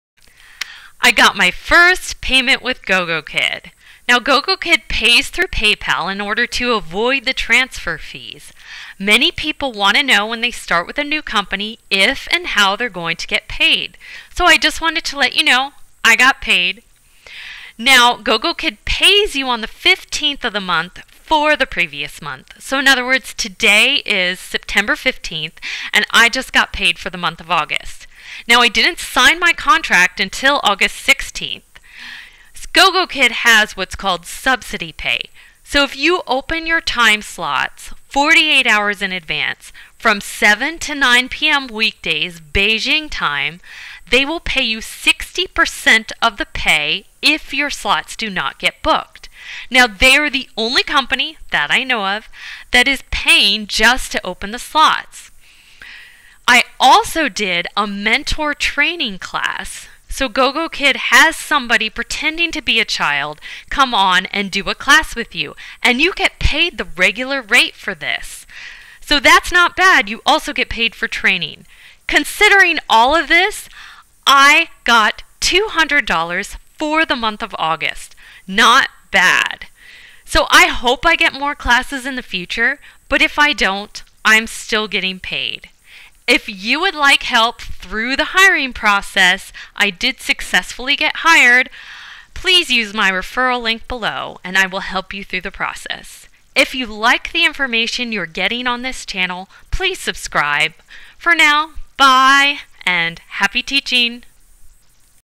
Gogokid: I got my 1st Paycheck
I got my VERY first paycheck at Gogokid. I describe how the payment system works, subsidy pay, getting paid for training classes, etc.